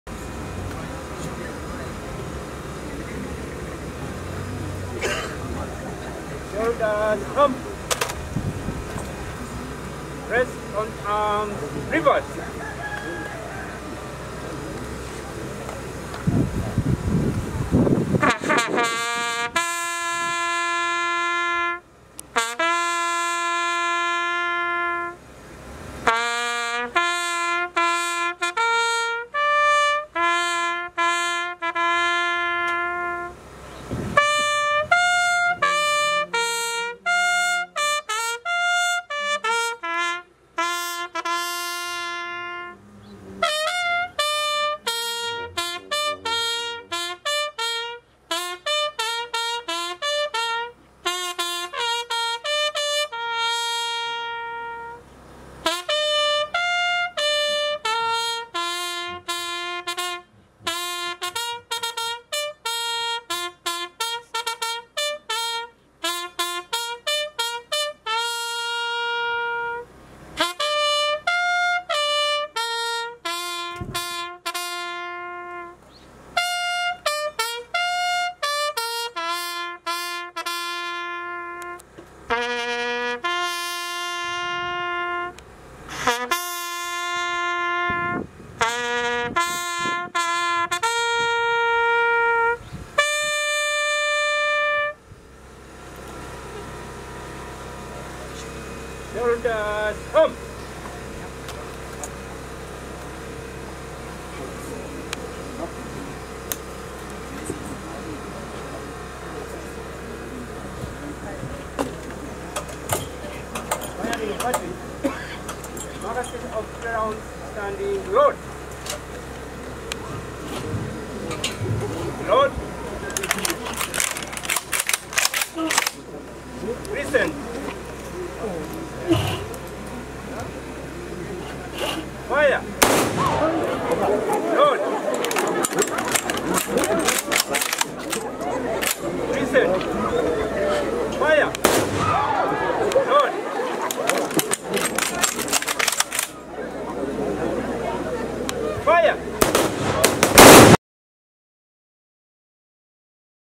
GUNSHOOT at a Police officer burial that Crushed my Camera😭😭😭😭😭😭😭😭😭😭
Livestream I Videography and Photography.